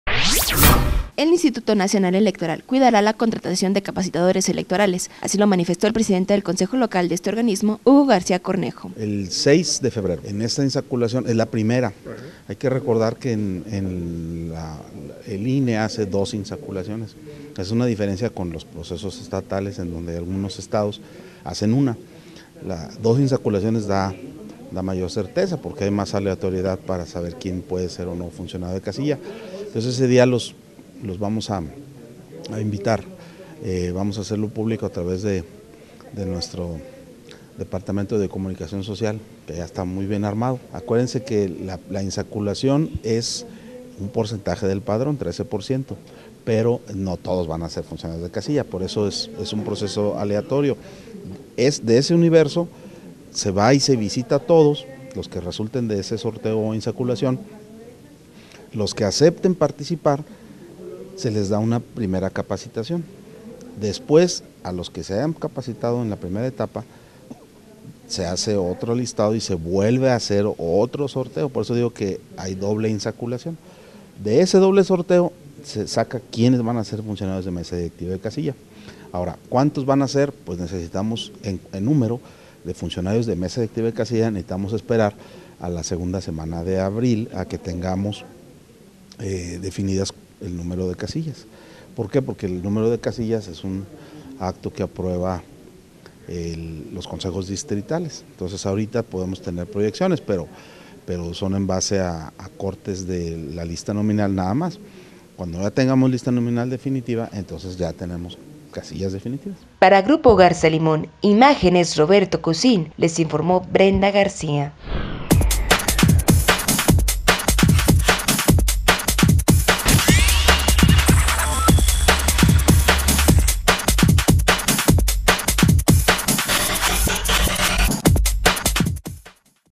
Cuidará INE contratación de capacitadores electorales
Cuidará INE contratación de capacitadores electorales. El Instituto Nacional Electoral (INE) cuidará la contratación de capacitadores electorales, manifestó el presidente del consejo local de este organismo Hugo García Cornejo.